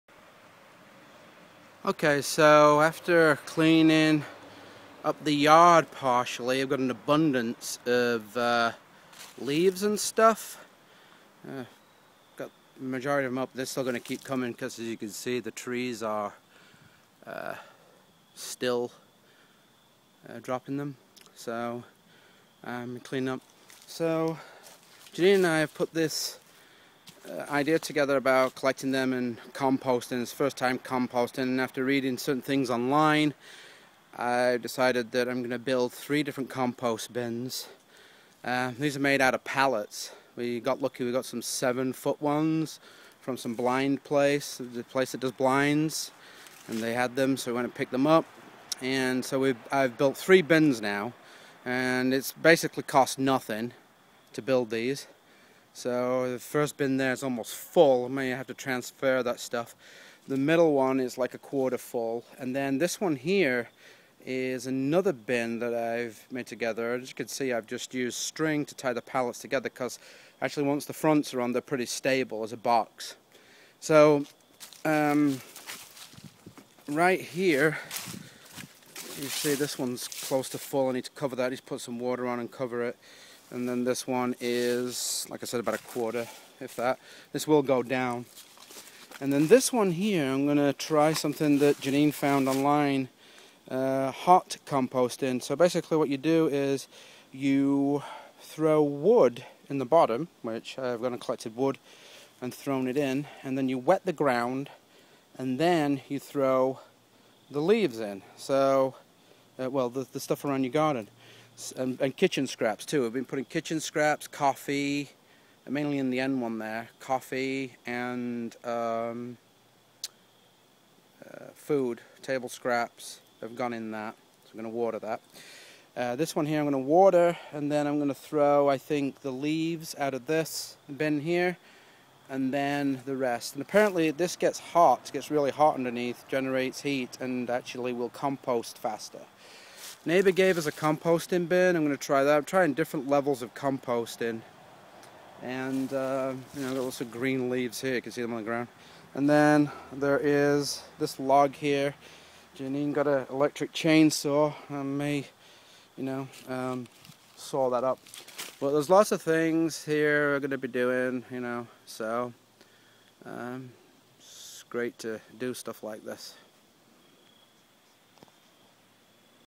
Composting
November 19, 2012 - The compost heap to the right has been sat for over a week prior to the hot composting technique in the far left bin.